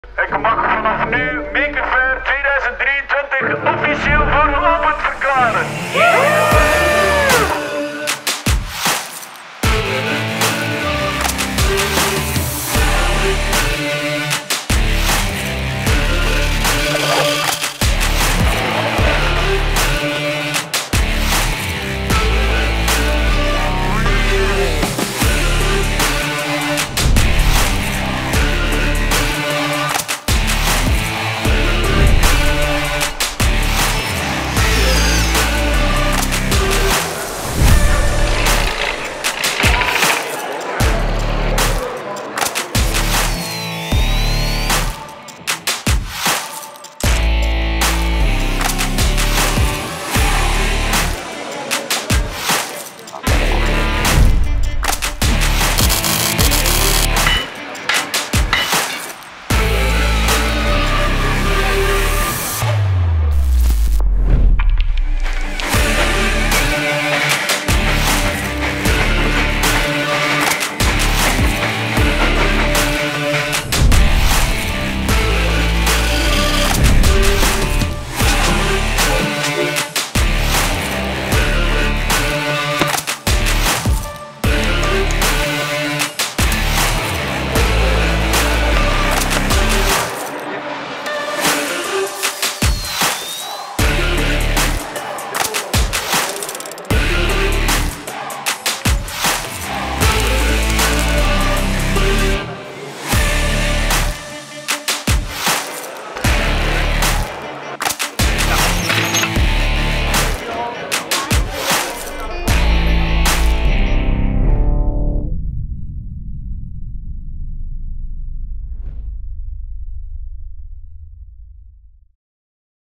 Dit was Maker Faire Gent 2023
Wat was me dat, zeg!
Op 28, 29 en 30 april werd de Chinastraat in Gent overrompeld door een enthousiast leger van makers die met heel passie hun werk lieten zien aan het grote publiek. Drie dagen feest! Meer dan 4000 bezoekers konden genieten van workshops, spektakel en show cases. Blokkeer 26, 27 en 28 april 2024 in je agenda want dan doen we het nog eens...

MAKER FAIRE GENT 2024:
Schrijf je nu alvast in op onze newsletter. Dan ben je als eerste op de hoogte van onze nieuwe editie